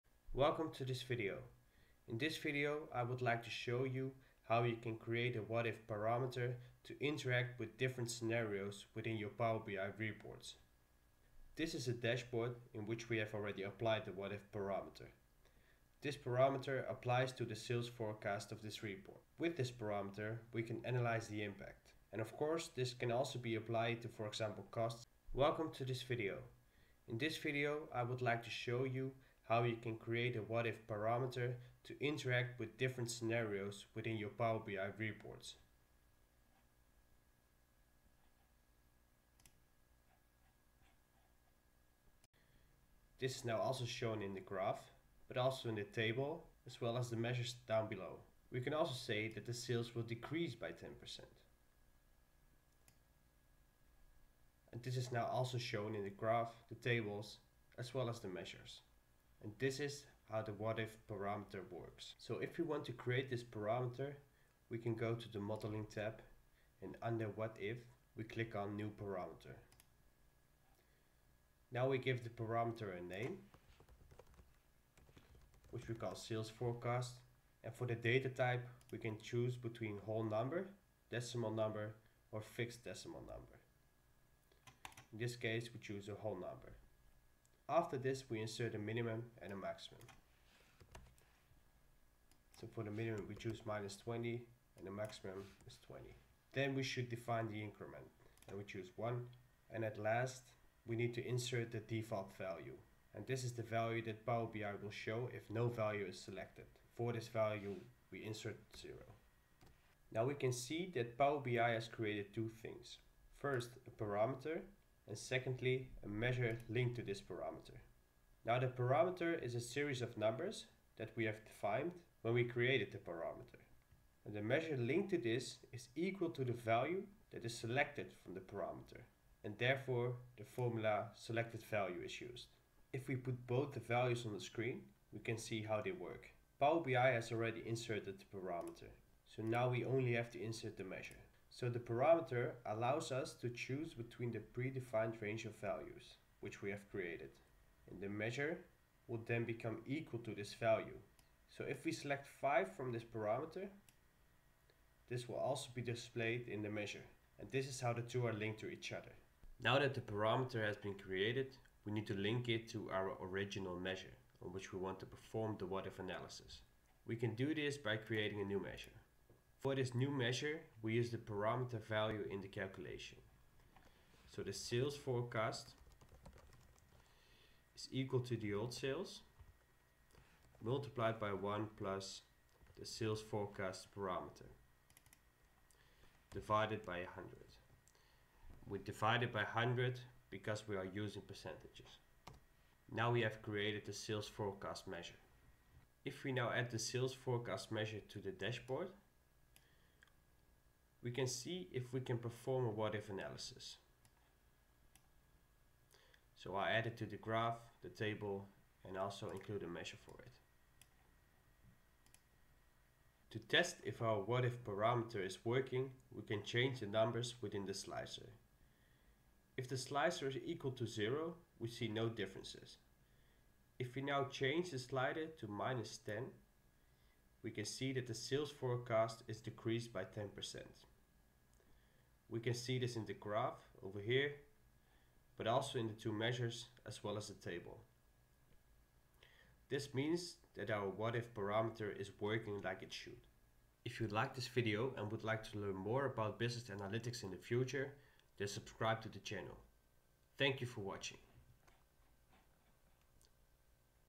How to create a scenario analysis in PowerBI
In this video you will learn how you can apply what if parameters to create a scenarion analysis im PowerBI.

The what if analysis allows users to change data according to predefined values. These values can then be linked to a measure that will show you different outputs depending on what you have selected.

Chapters
00:00 - 00:11 : Intro
00:11 – 01:00 Example
01:00 – 02:49 Create parameter
02:49 – 03:19 Link parameter
03:19 – 04:03 Demo of scenario analysis
04:03 – 04:14 Outro